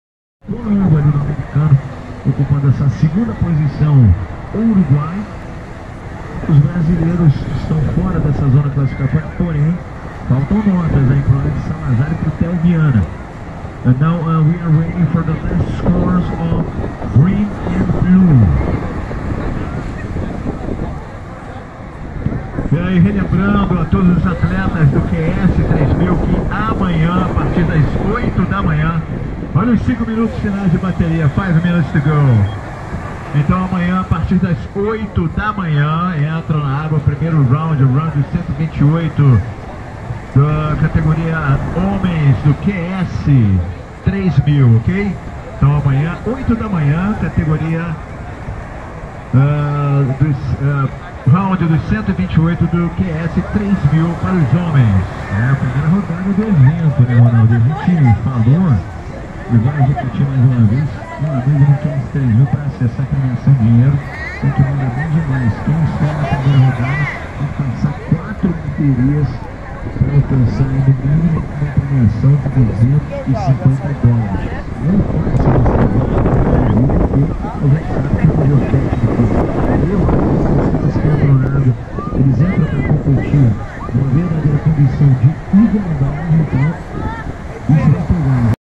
SAQUAREMA SURF FESTIVAL ,PRIMEIRO DIA 15-11-2021
Começou!!
Hoje é o primeiro dia do Saquarema Surf Festival. O torneio, que está sendo realizado na Praia de Itaúna, reúne grandes nomes do surf nacional e internacional.